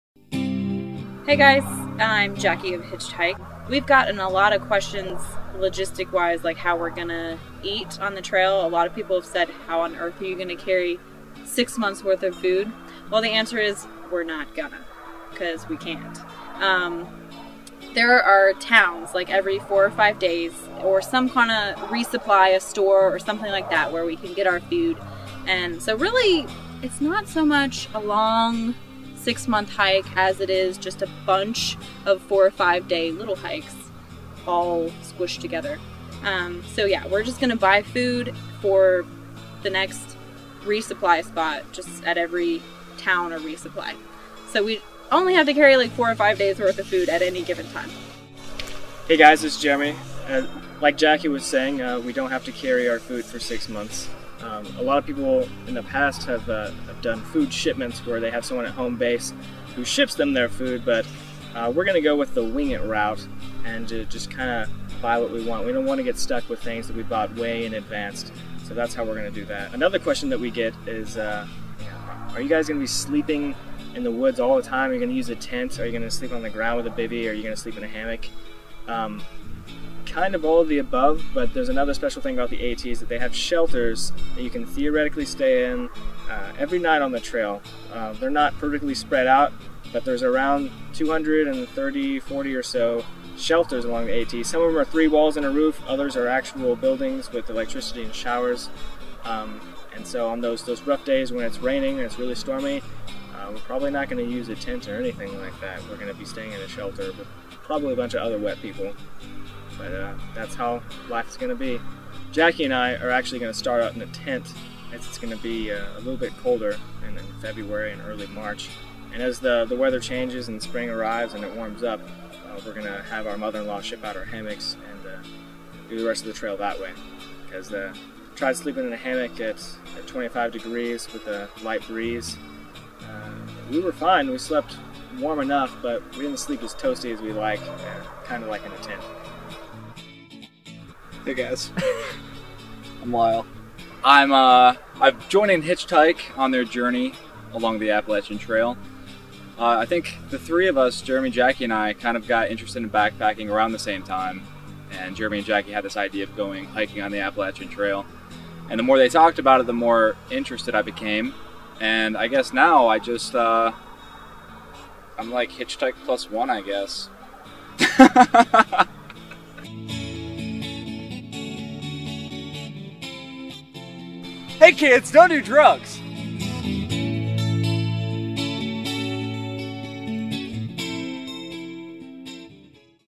Appalachian Trail Logistics + Lyle
Jacki and I discuss a few questions that we're frequently asked about our thru-hike of the trail and Lyle officially introduces himself.

We'll do our best to answer.  Keep in mind, we haven't actually hiked the trail yet.  We will start Feb 27th.

Check out Whiteblaze.net and Appalachiantrail.org for all the information you could possibly imagine and more.

As always, you can follow us on HitchedHike.com